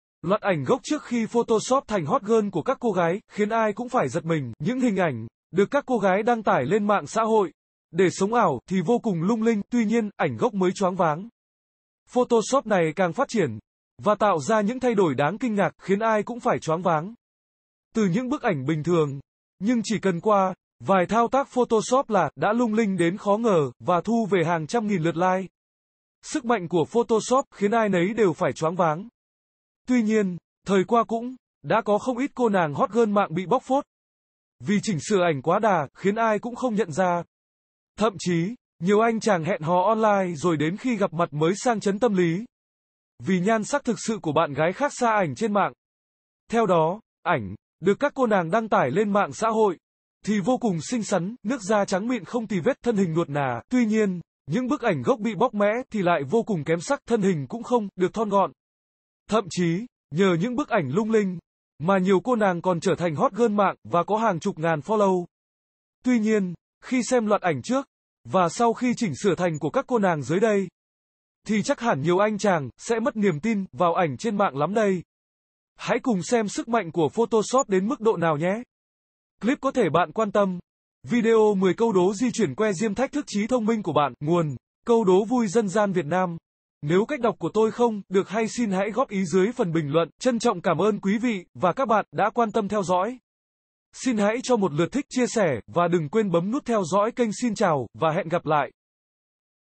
Loạt ảnh gốc trước khi photoshop thành 'hot girl' của các cô gái khiến ai cũng phải 'giật mình'a
Tải Game Android hay Miễn Phí Đăng kí kênh xem tin tức mới nhất Những hình ảnh được các cô gái đăng tải lên mạng xã hội để 'sống ảo' thì vô cùng lung linh, tuy nhiên, ảnh gốc mới choáng váng.Photoshop này càng phát triển vàtạo ra những thay đổi đáng kinh ngạc khiến ai cũng phải "choáng váng"
Từ những bức ảnh bình thường nhưng chỉ cần qua vài thao tác photoshop là đã lung linh đến khó ngờ và thu về hàng trăm nghìn lượt like.Sức mạnh của photoshop khiến ai nấy đều phải choáng váng.Tuy nhiên, thời qua cũng đã có không ít cô nàng "hot girl" mạng bị "bóc phốt" vì chỉnh sửa ảnh quá đà khiến ai cũng không nhận ra
Thậm chí, nhiều anh chàng hẹn hò online rồi đến khi gặp mặt mới "sang chấn tâm lý" vì nhan sắc thực sự của bạn gái khác xa ảnh trên mạng.Theo đó, ảnh được các cô nàng đăng tải lên mạng xã hội thì vô cùng xinh xắn, nước da trắng mịn không tì vết, thân hình nuột nà
Tuy nhiên, những bức ảnh gốc bị "bóc mẽ" thì lại vô cùng kém sắc, thân hình cũng không được thon gọn,..Thậm chí, nhờ những bức ảnh lung linh mà nhiều cô nàng còn trở thành "hot girl mạng" và có hàng chục ngàn follow.Tuy nhiên, khi xem loạt ảnh trước và sau khi chỉnh sửa thành của các cô nàng dưới đây thì chắc hẳn nhiều anh chàng sẽ mất niềm tin vào ảnh trên mạng lắm đây.Hãy cùng xem sức mạnh của photoshop đến mức độ nào nhé.Clip có thể bạn quan tâm:
- Video 10 câu đố di chuyển que diêm thách thức trí thông minh của bạn
Nguồn: Câu Đố Vui Dân Gian Việt Nam.